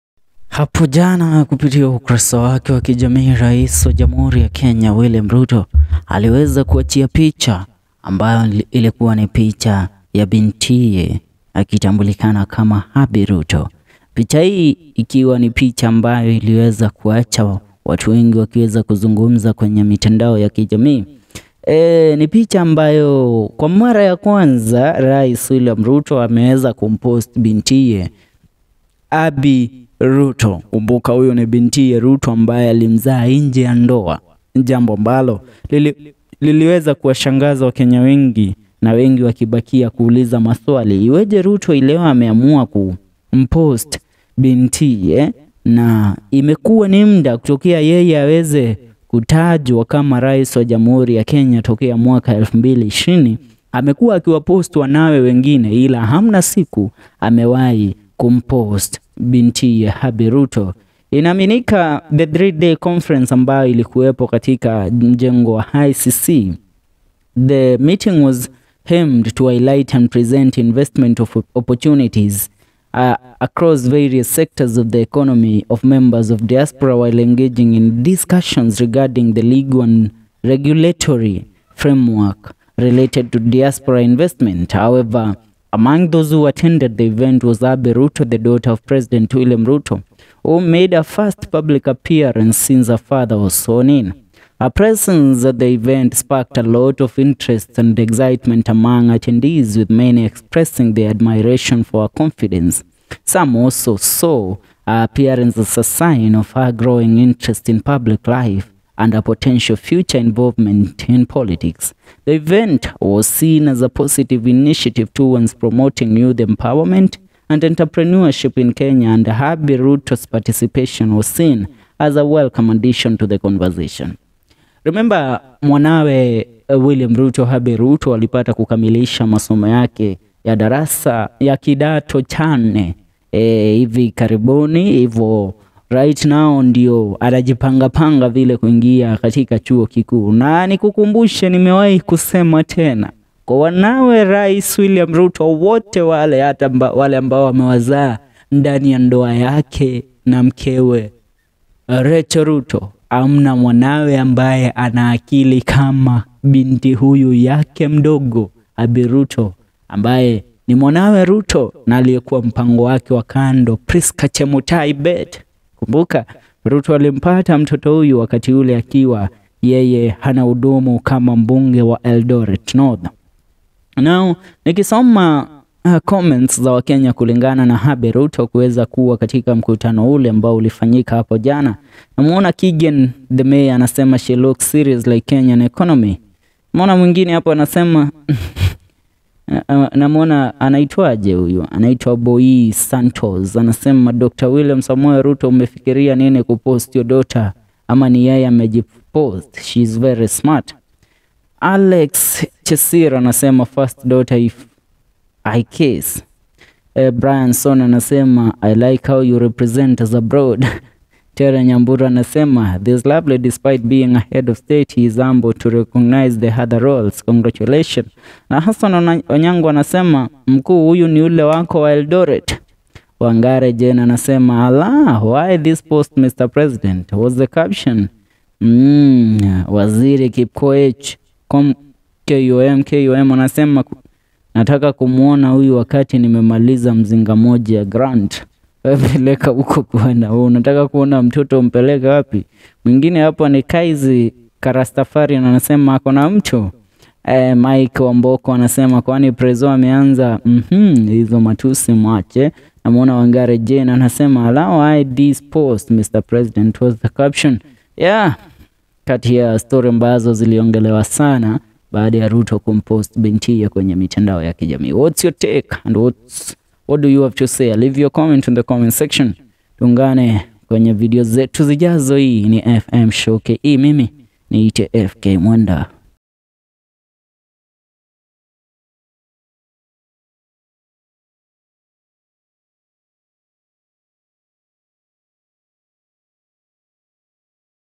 PRESIDENT RUTO FOR THE FIRST TIME POST HIS DAUGHTER ABBY RUTO BORN OUT OF WEDLOCK KENYAS REACT!
PRESIDENT RUTO AS LEFT KENYAS TALKING ON SOCIAL MEDIA AFTER SHARING A PICTURE OF HIS BEAUTIFUL DAUGHTER ABBY RUTO